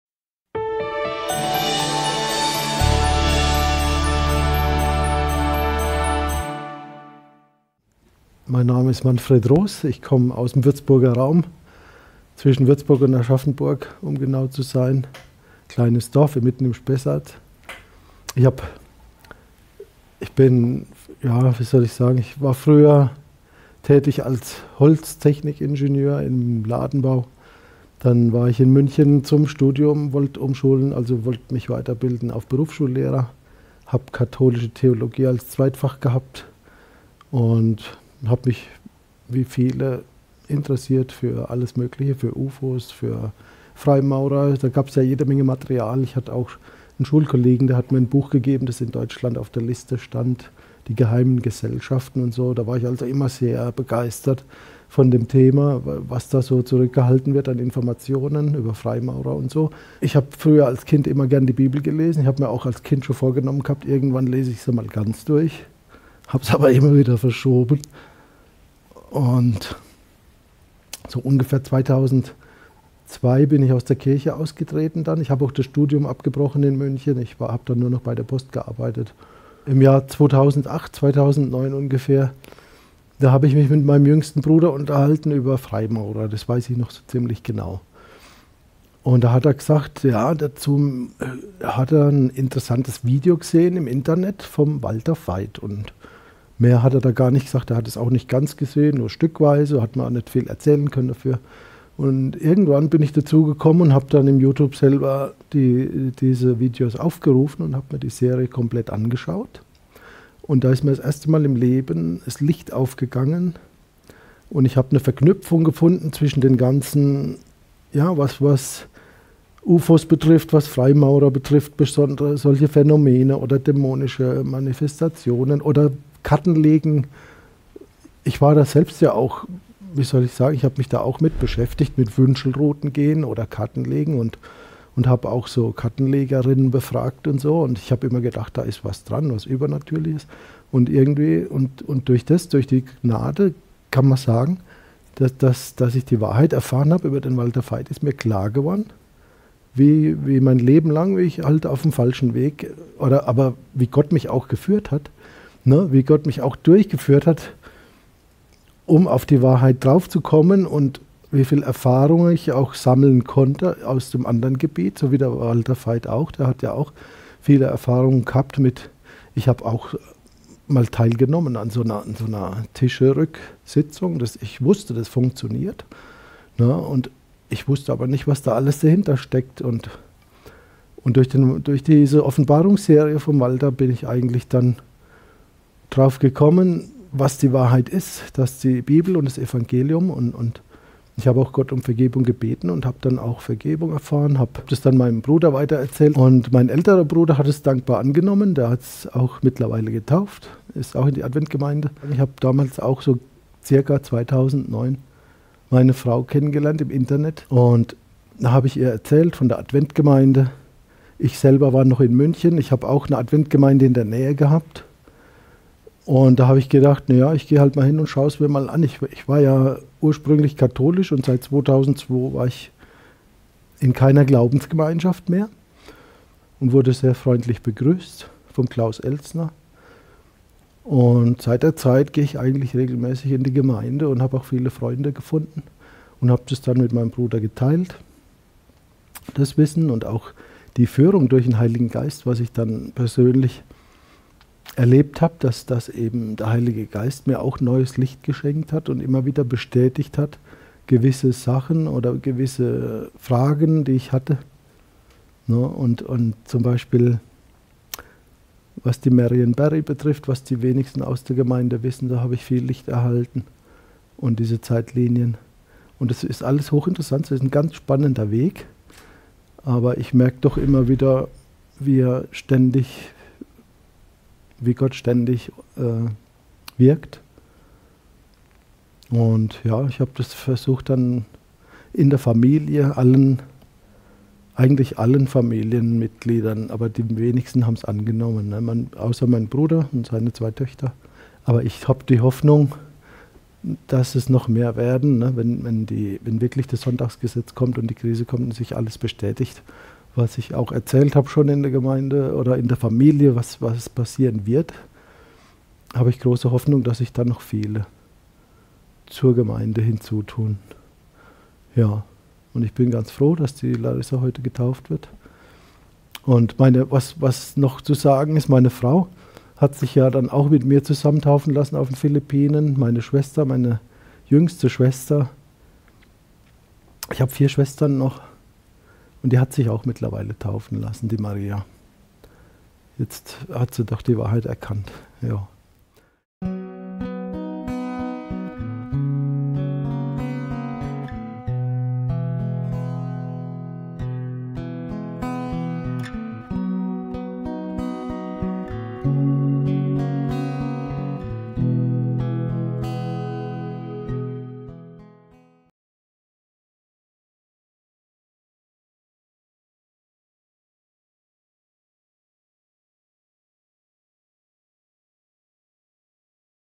Lebenszeugnisse, 01. Eine Familie lernt Jesus kennen, Teil 1
Wie eine Familie den Weg zu Jesus kennenlernt.